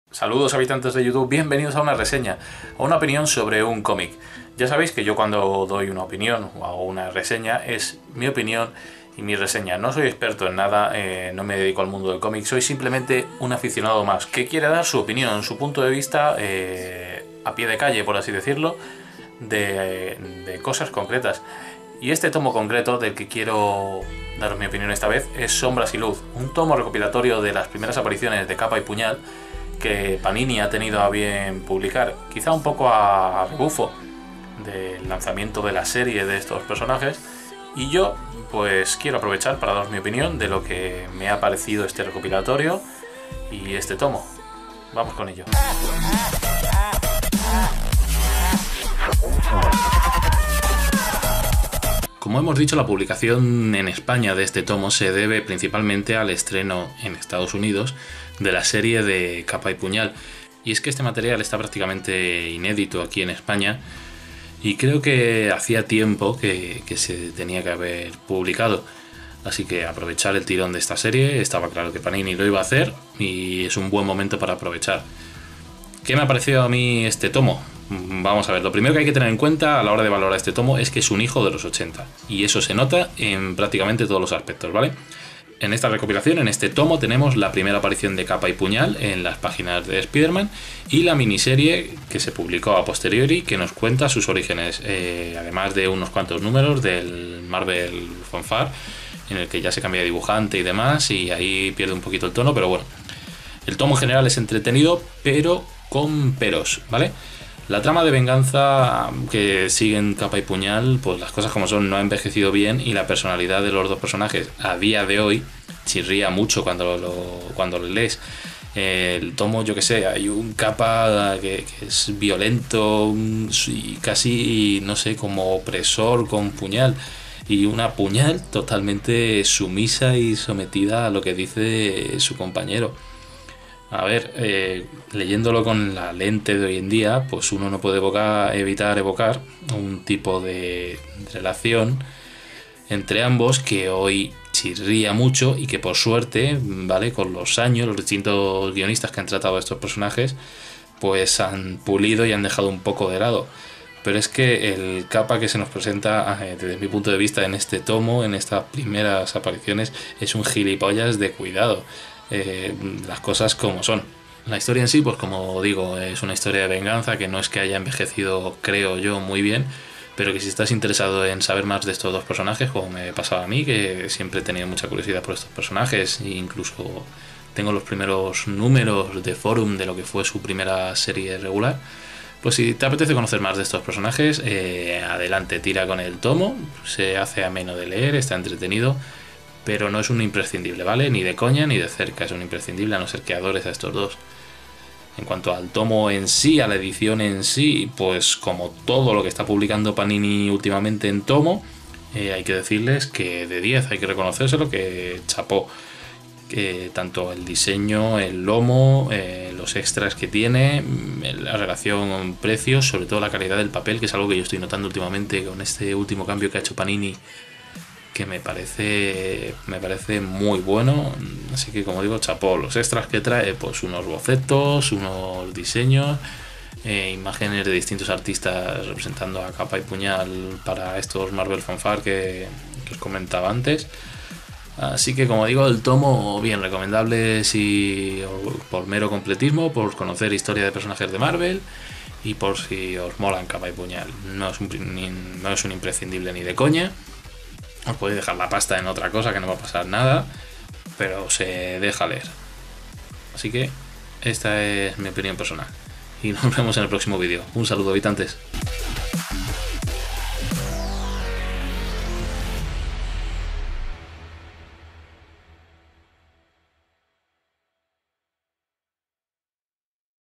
Capa y Puñal "Sombras y Luz" - Jack´s Bullpen Vol.02
Con motivo del estreno de la serie Capa y Puñal (Cloak and Dagger) Panini ha recopilado en un tomo las primeras apariciones de los persones, con sus origenes incluidos.
Esta es mi opinión del tomo y lo que contiene.